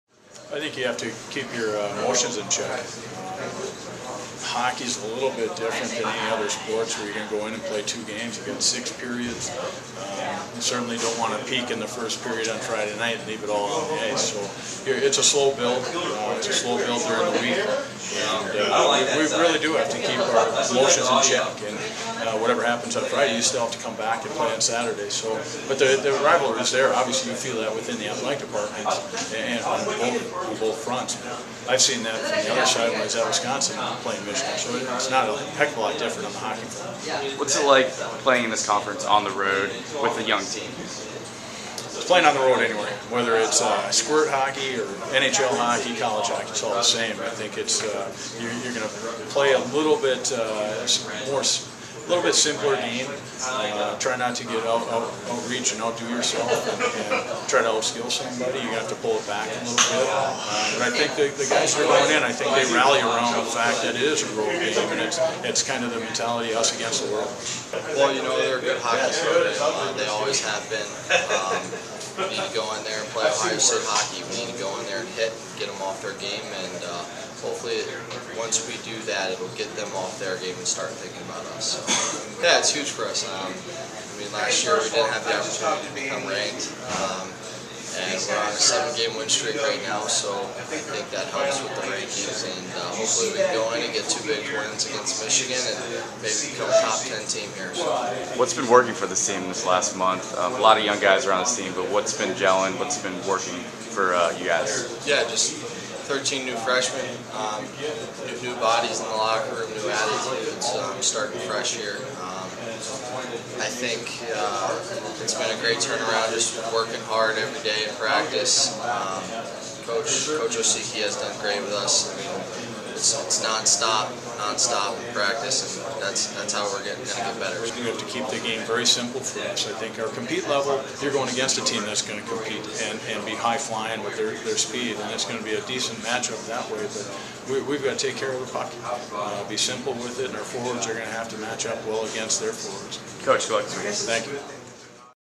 Ohio State Men's Hockey - Coach Osiecki and Chris Crane Preview Michigan Series
Ohio State Men's Hockey coach Mark Osiecki and sophomore Chris Crane preview the series at Michigan Nov. 18-19, 2011